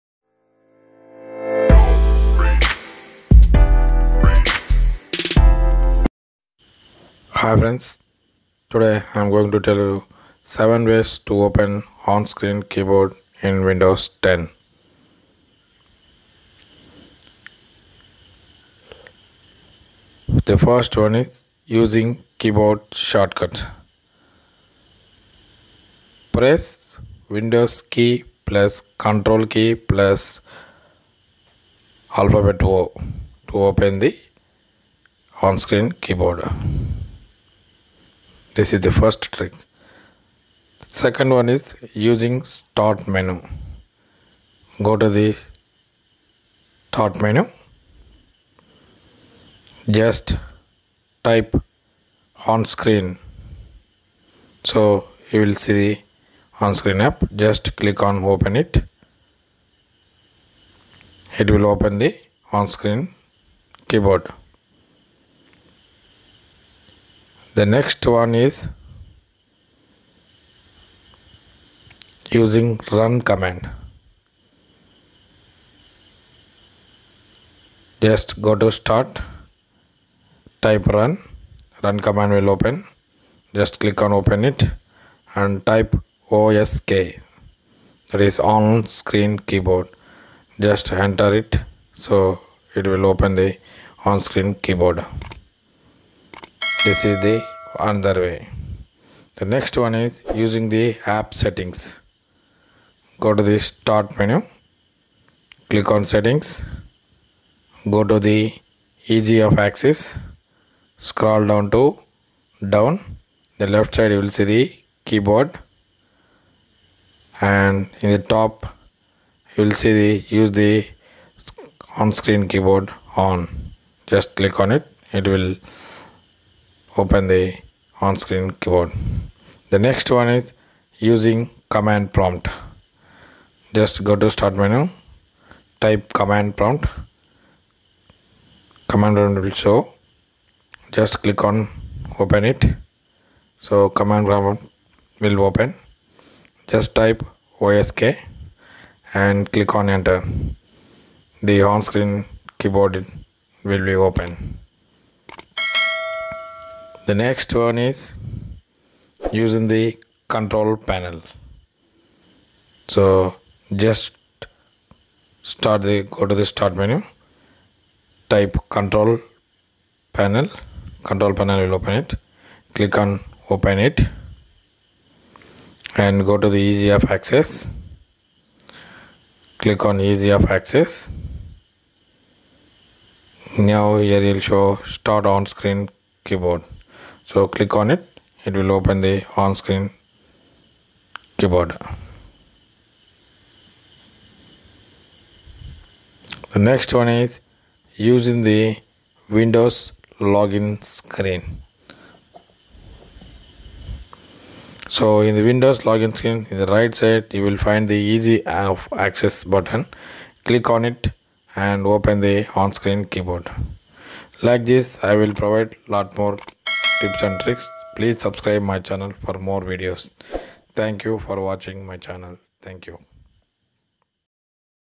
7 Ways To Open On Screen Keyboard in Windows 10 [On Screen Keyboard]
7 Ways To Open On Screen Keyboard  in Windows 10,we can open on screen keyboard in 7 Ways for typing in laptops or desktops on Windows 10,Here are the 7 Ways to open on screen keyboard

         1.Using Keyboard Shortcut  i.e  Windows key + Ctrl + O
         2.Using Start Menu
         3.Using Run Command
         4.Using App Settings
         5.Using Command Prompt
         6.Using Control Panel
         7.Using Windows Login Screen